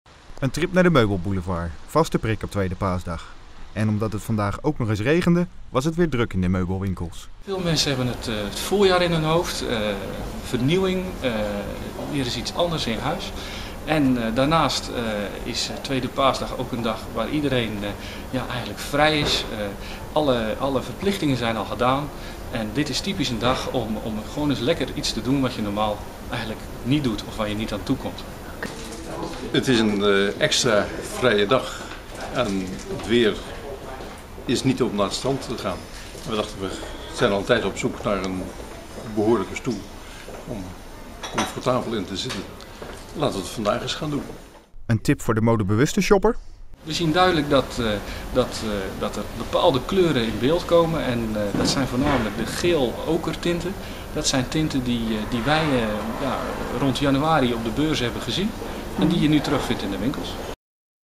Grote toeloop meubelzaken op Tweede Paasdag
Tweede Paasdag is de nationale meubelboulevarddag.
 Bij de entree van meubelboulevard de Mortiere in Middelburg bijvoorbeeld was het filerijden. De verkopers hebben het razend druk.

Zelfs tuinmeubels gingen met het gure paasweer grif over de toonbank.

Ook de meubelwinkels in de kleinere plaatsen deden goede zaken.

Bij Woontrend Zeeland in Koudekerke verwacht eigenaar Erik de Wee een grote toeloop. "De mensen hebben gewoon een vrije dag over. Ze hebben vandaag geen verplichtingen meer, alles is gedaan wat ze moesten doen, en dan is het een mooi moment om samen meubels te kopen, of een stoel op bank op maat bijvoorbeeld te testen." Maar er is meer aan de hand waarom Tweede Paasdag aanslaat. "Bij het voorjaar hoort al jarenlang dat er goed wordt schoongemaakt. De voorjaarsschoonmaak luidt een nieuwe start in en daar horen nieuwe spullen bij", zegt De Wee.